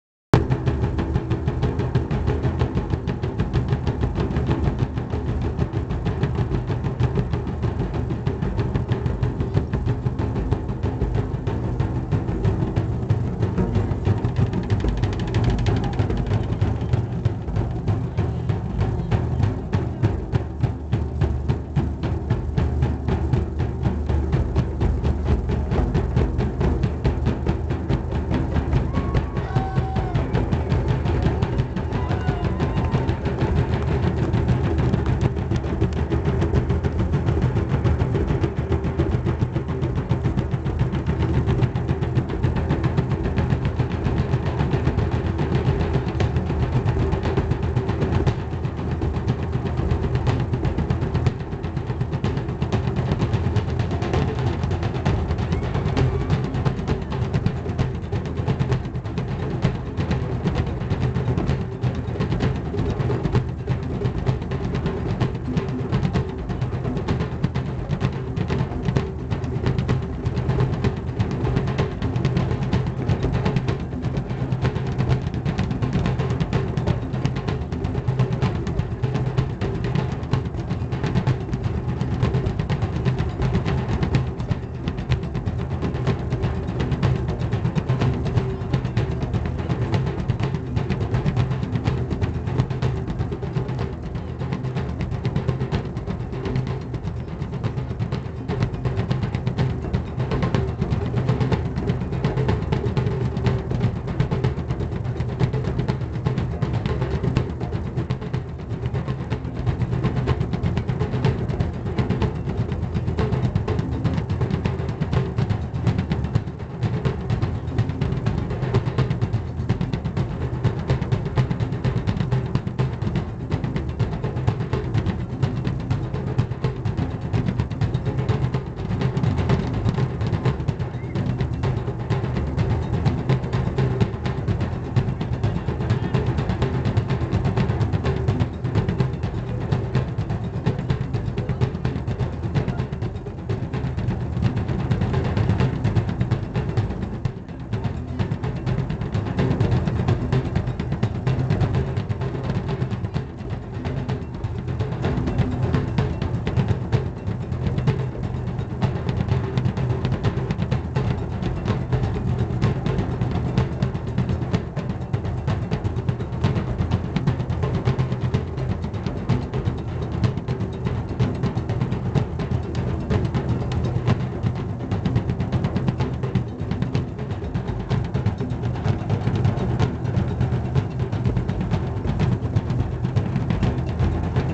Drum Jam
Drum jam at the Arizona Renaissance Festival, end of day.  I'm the one with the flowing green scarves, in case you wanted to know...